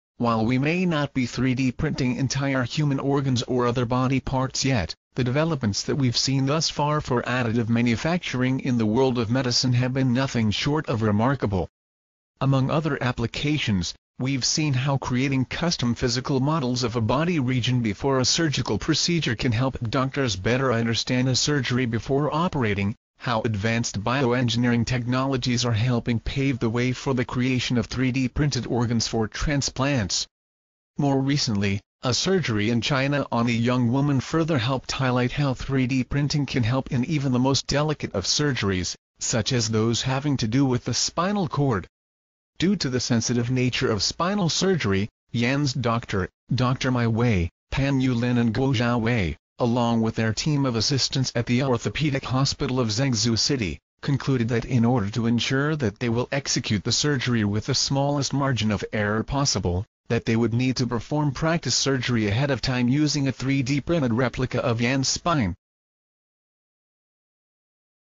Chinese doctors use 3D printed spine of patient to help in delicate spinal surgery
Best 3D Printing Gadgets
While we may not be 3D printing entire human organs or other body parts yet, the developments that we’ve seen thus far for additive manufacturing in the world of medicine have been nothing short of remarkable.
Among other applications, we’ve seen how creating custom physical models of a body region before a surgical procedure can help doctors better understand a surgery before operating, how advanced bioengineering technologies are helping pave the way for the creation of 3D printed organs for transplants.
More recently, a surgery in China on a young woman further helped highlight how 3D printing can help in even the most delicate of surgeries - such as those having to do with the spinal cord.
Due to the sensitive nature of spinal surgery, Yan's doctor, Dr. Mei Wei, Pan Yu Lin and Guo Xiao Wei - along with their team of assistants at the Orthopaedic Hospital of Zhengzhou City - concluded that in order to ensure that they will execute the surgery with the smallest margin of error possible, that they would need to perform practice surgery ahead of time using a 3D printed replica of Yan's spine.